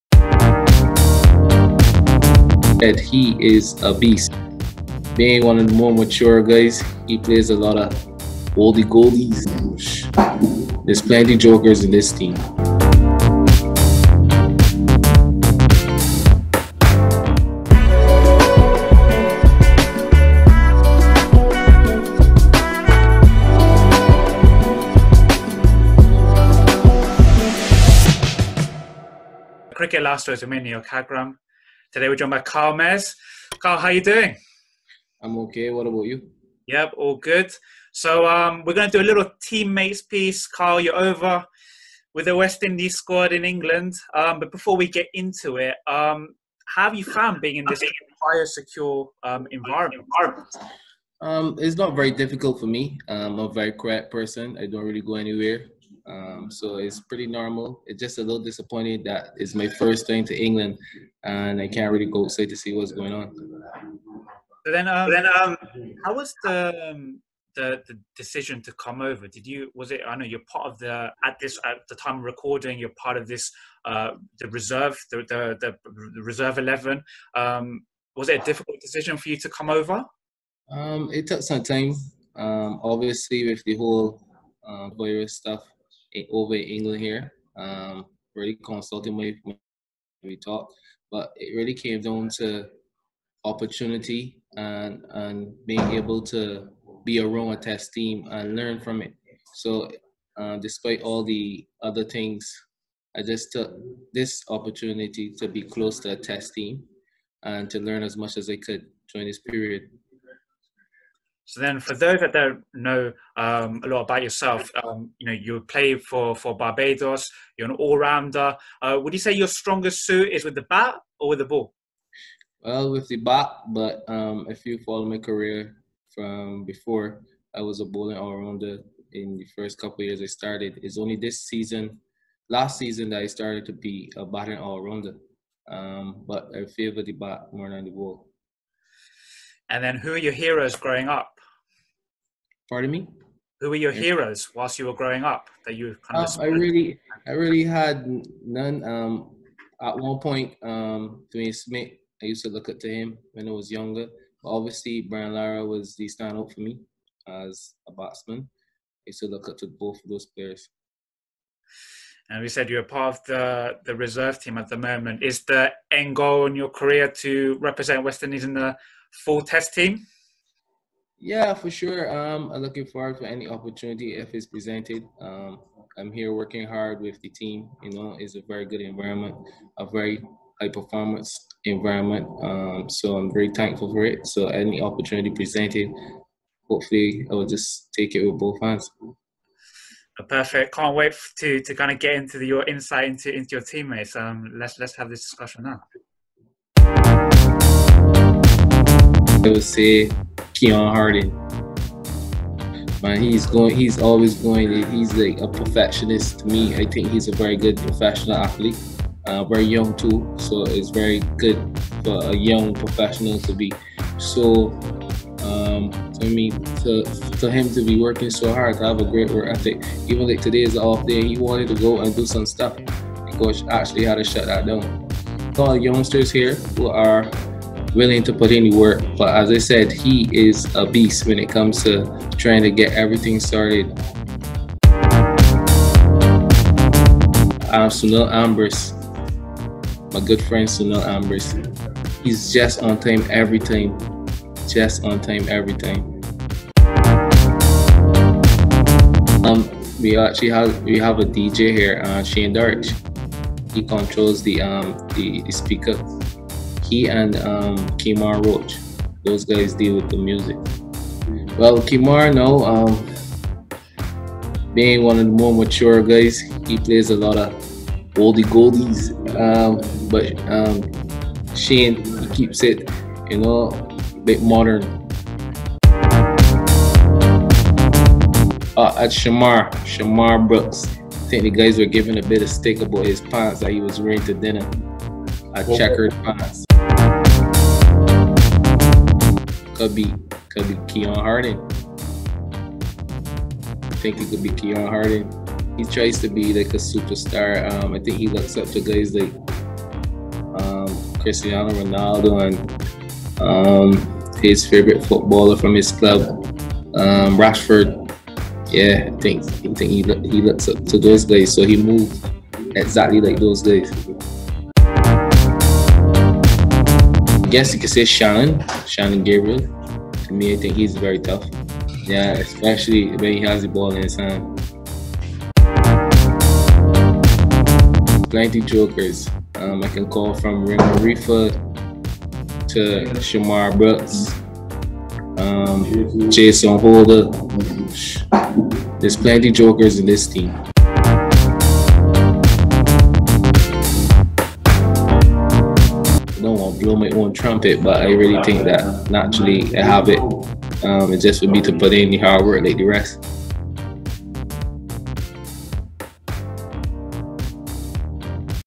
England vs West Indies: Kyle Mayers talks Windies teammates ahead of Test series against England.
All-rounder Kyle Mayers discusses tour of England so far, and his West Indies teammates.

Interview by Neel Khagram